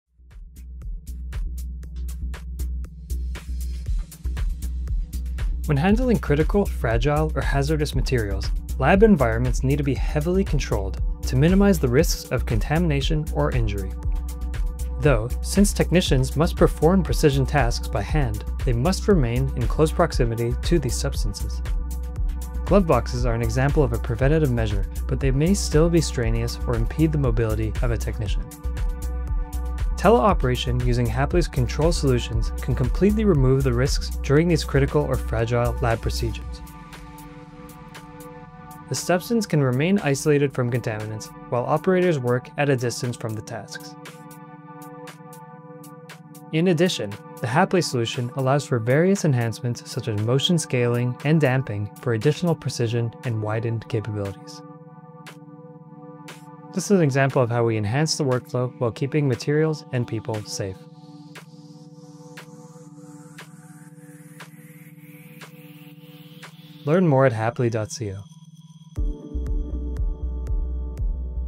Why Lab Work Is Safer and Smarter With Haptic Control
Discover how Haply’s Inverse3 controller is transforming laboratory workflows with safe, precise teleoperation. In this video, we demonstrate how technicians can pipette remotely, keeping both people and samples protected from contamination, fatigue, or exposure.

Unlike glove boxes or traditional lab hoods, Haply’s haptic interface enables real-time control with intuitive motion scaling and fine-tuned precision. That means fewer risks, more flexibility, and no need to compromise on workflow quality.

Haply Robotics | Bringing Touch to Technology